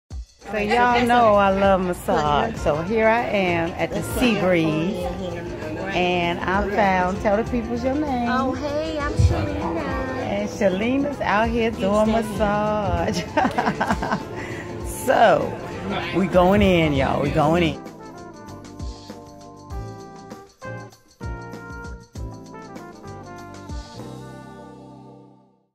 What do you love? Daily Create Video Assignment
For my ILT5340 class I chose this video assignment for my daily create.  Choose something that you love and make a video of it!  I chose one of my first loves MASSAGES!